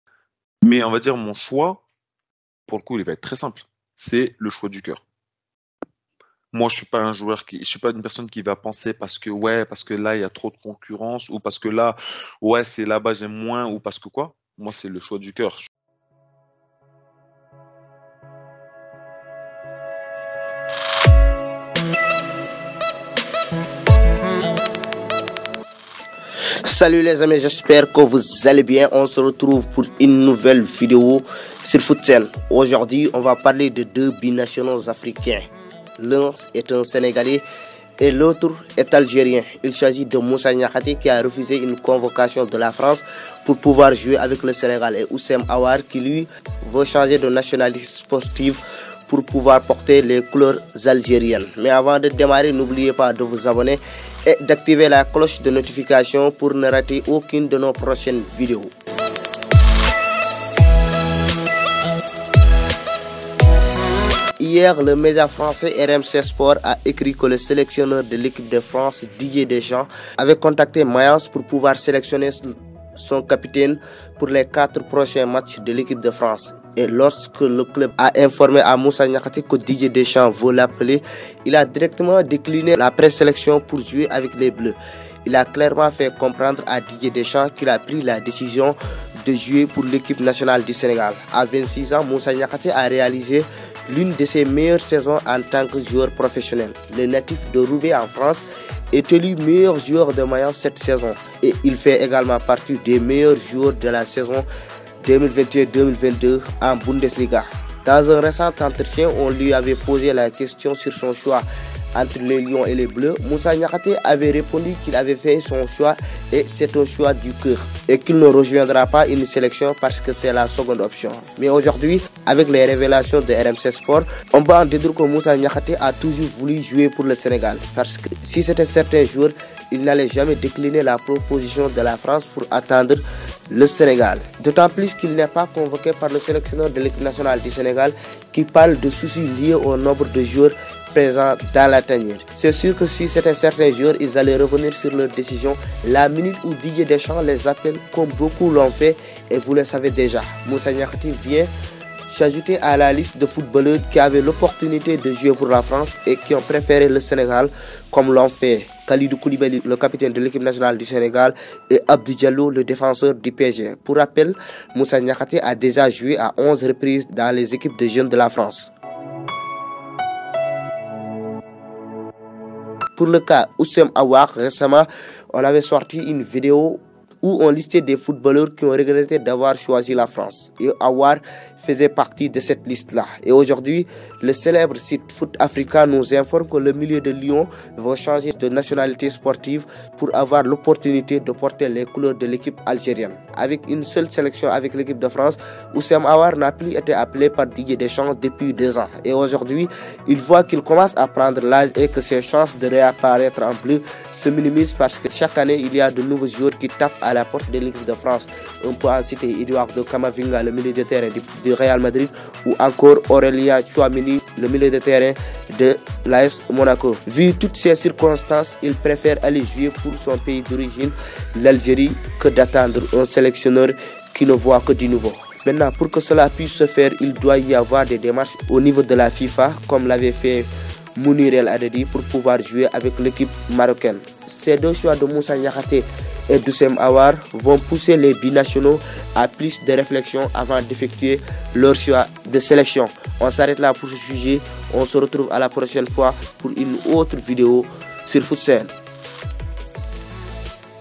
Moussa Niakhaté refuse la France/ Houssem Aouar veut changer de nationalité sportive ( BINATIONAUX).
En marge des quatre prochaines rencontres de l'équipe de France, Didier Deschamps voulait appeler le défenseur de Mayence MOUSSA NIAKHATE. Mais ce dernier a refusé et a fait comprendre au coach qu'il va jouer pour l'équipe nationale du Sénégal.
De l'autre côté, Hossem Aouar qui compte qu'une seule sélection avec l'équipe de France veut changer de nationalité pour pouvoir jouer avec l'équipe algérienne.